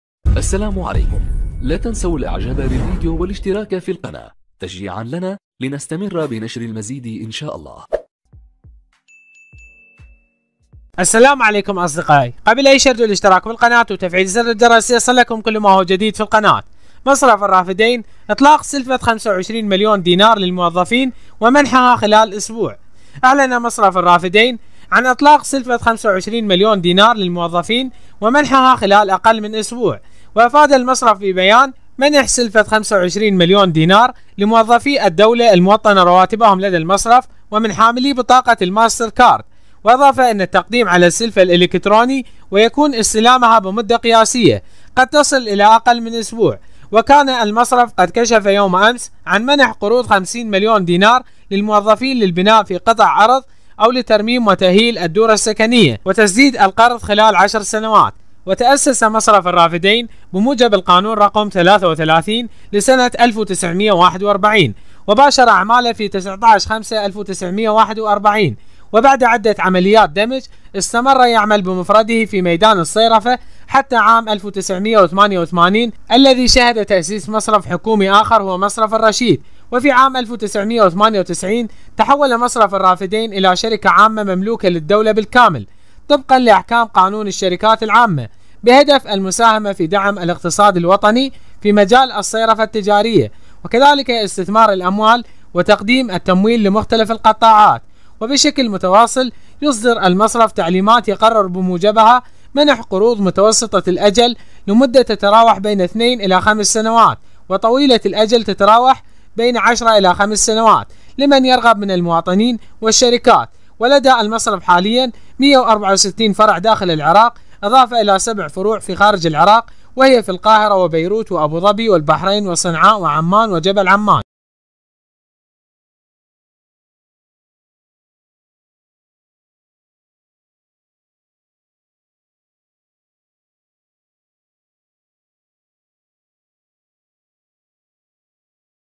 مصرف الرافدين: إطلاق سلفة 25 مليون دينار للموظفين ومنحها خلال أسبوع
أعلن مصرف الرافدين، اليوم الثلاثاء، (15 أيلول 2020)، عن إطلاق سلفة 25 مليون دينار للموظفين ومنحها خلال أقل من أسبوع.

وأفاد المصرف في بيان  "منح سلفة 25 مليون دينار لموظفي الدولة الموطنة رواتبهم لدى المصرف ومن حاملي الماستر كارد".

وأضاف أن التقديم على السلفة إلكتروني ويكون استلامها بمدة قياسية قد تصل إلى أقل من أسبوع.

وكان المصرف قد كشف أمس عن منح قروض 50 مليون دينار للموظفين للبناء في قطعة أرض أو لترميم وتأهيل الدور السكنية لهم وتسديد القرض خلال 10 سنوات.

وتأسس مصرف الرافدين بموجب القانون رقم (33) لسنة 1941 وباشر أعماله في 19/ 5/ 1941 وبعد عدة عمليات دمج، استمر يعمل بمفرده في ميدان الصيرفة حتى عام 1988 الذي شهد تأسيس مصرف حكومي آخر هو مصرف الرشيد.

وفي عام 1998 تحول المصرف الرافدين إلى شركة عامة مملوكة للدولة بالكامل طبقاً لاحكام قانون الشركات العامة "بهدف المساهمة في دعم الاقتصاد الوطني في مجال الصيرفة التجارية واستثمار الاموال وتقديم التمويل لمختلف القطاعات".

وبشكل متواصل، يصدر المصرف تعليمات يقرر بموجبها منح قروض متوسطة الأجل لمدد تتراوح بين 2-5 سنوات وطويلة الأجل لمدد تتراوح بين 5-10 سنوات لمن يرغب من المواطنين والشركات.

ولدى المصرف حالياً 164 فرعاً داخل العراق إضافة إلى 7 فروع في الخارج وهي: القاهرة، بيروت، أبو ظبي، البحرين، صنعاء، عمان، جبل عمان.